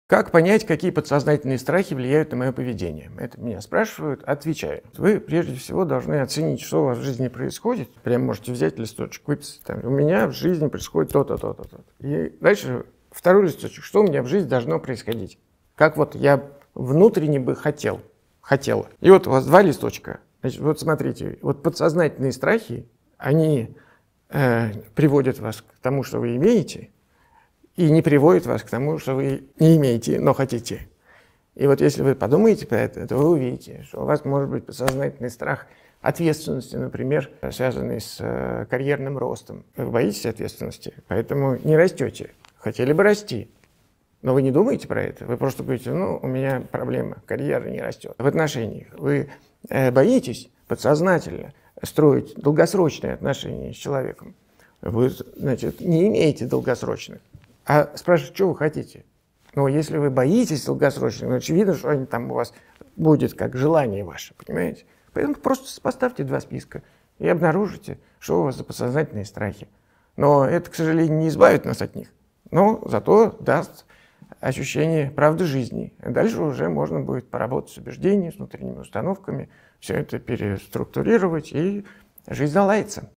Как понять свои подсознательные страхи? | Андрей Курпатов
kurpatov_official: Подсознательные страхи — это те страхи, которые влияют на ваше поведение, но вы не осознаёте, что ваши действия продиктованы страхом.

Страх требует от нас бегства. И, думаю, многие сталкивались с ситуациями, когда избегали чего-то — каких-то поступков или решений, но особенно и не задумывались почему.

Кто-то избегает близких или эмоциональных отношений, хотя страдает от одиночества. Почему?

Кто-то уклоняется от карьерного роста и повышения по службе, но при этом переживает, что его не ценят.

Кто-то понимает, что его отношения с партнёром подошли к концу, но почему-то не говорит ему об этом.

Кто-то давно хочет сменить работу, но не меняет...

Как правило, за этим странным, с точки зрения логики, поведением скрывается именно подсознательный страх. И подобные страхи незаметно, но сильно портят нам жизнь.

К сожалению, от таких страхов не так-то просто избавиться, и главная проблема в том, что такой страх трудно осознать.